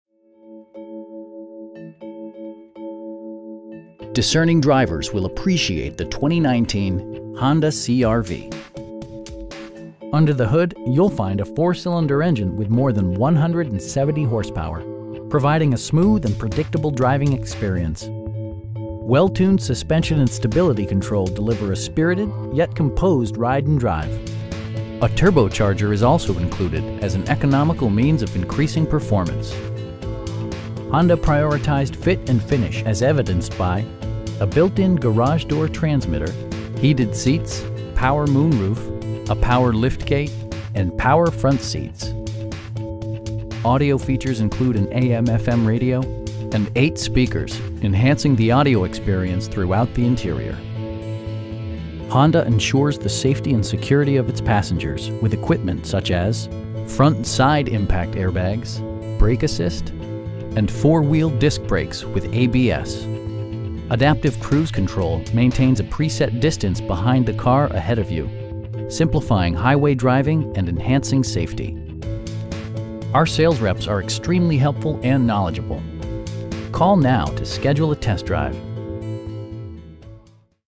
2019 Honda CR-V EX-L 2WD in San Bruno, CA 94066
Discerning drivers will appreciate the 2019 Honda CR-V. Under the hood you'll find a 4 cylinder engine with more than 170 horsepower, providing a smooth and predictable driving experience. Well tuned suspension and stability control deliver a spirited, yet composed, ride and drive A turbocharger is also included as an economical means of increasing performance. Honda prioritized fit and finish as evidenced by: a built-in garage door transmitter, heated seats, front fog lights, power moon roof, a power liftgate, remote keyless entry, and power front seats. Features such as automatic climate control and leather upholstery prove that economical transportation does not need to be sparsely equipped. Audio features include an AM/FM radio, steering wheel mounted audio controls, and 8 speakers, providing excellent sound throughout the cabin. Honda ensures the safety and security of its passengers with equipment such as: head curtain airbags, front side impact airbags, traction control, brake assist, a panic alarm, and 4 wheel disc brakes with ABS. Adaptive cruise control maintains a pre set distance behind the car ahead of you, simplifying highway driving and enhancing safety. Our sales reps are extremely helpful & knowledgeable. We'd be happy to answer any questions that you may have.